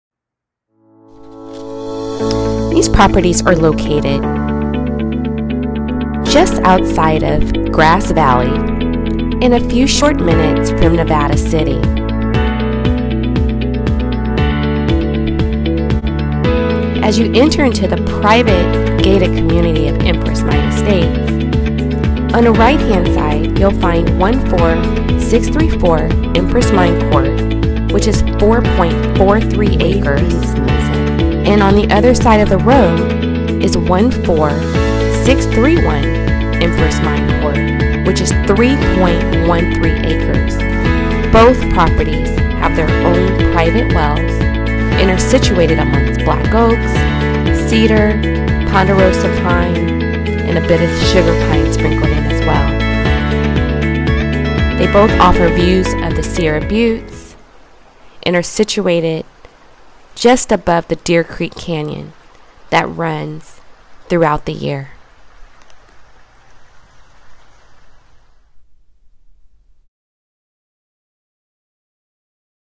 Drone Video | Land For Sale
LandForSale
14634 & 14631 Empress Mine Ct., Nevada City, California

4.43 acres & 3.13 Acres

Build your dreamhome or maybe even a tinyhouse on a beautiful piece of land on a private gated road with views of the SierraButtes and DeerCreekCanyon Bring the kids horses 🐎 chickens 🐔 big boy and girl Toys trailers plant a garden and watch it grow from your porch